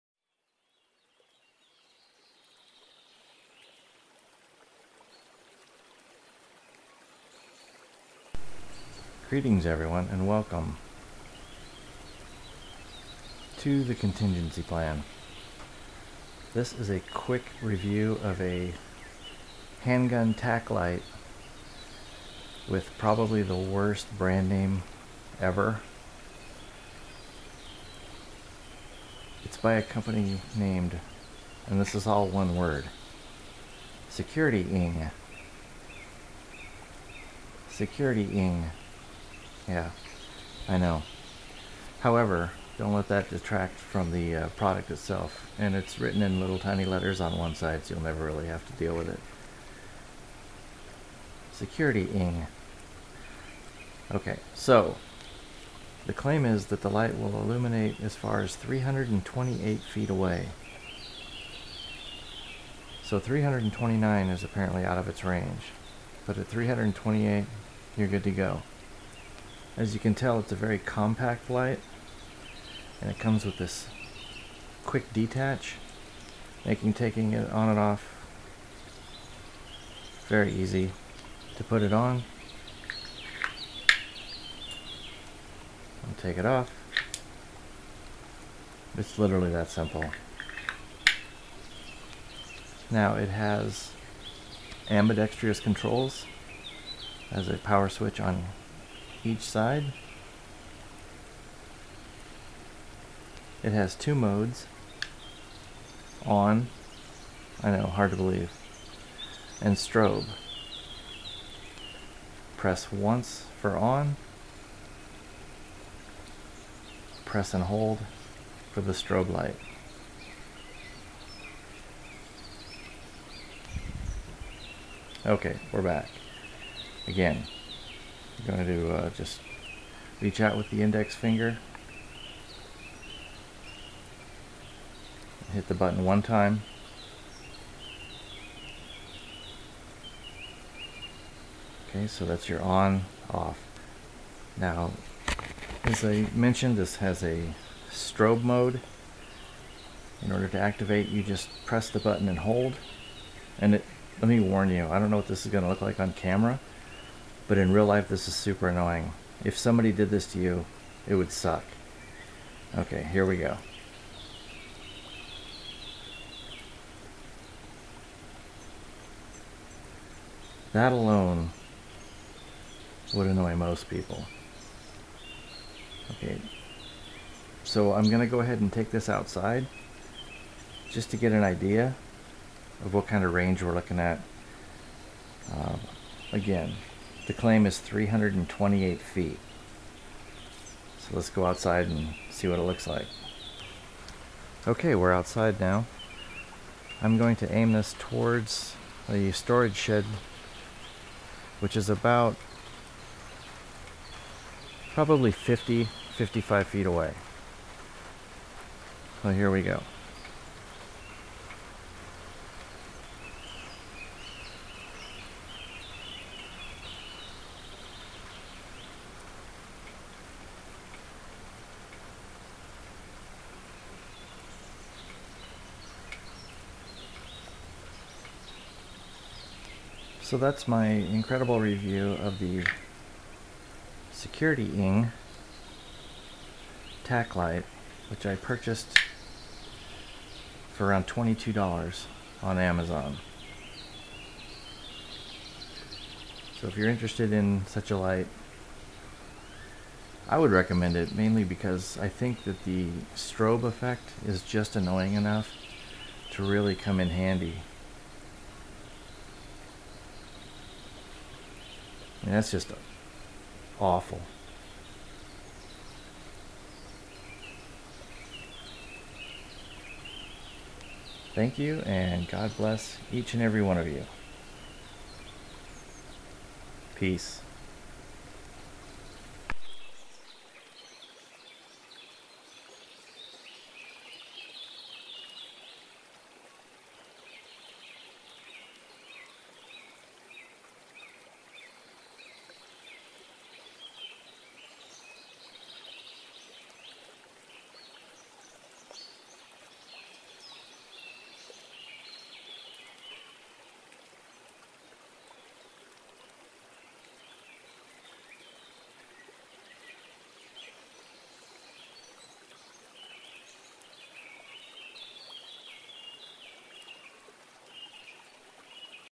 Tactical Handgun Light Review
¯\_(ツ)_/¯
Review of the SecurityIng tactical handgun light.
Be kind, rewind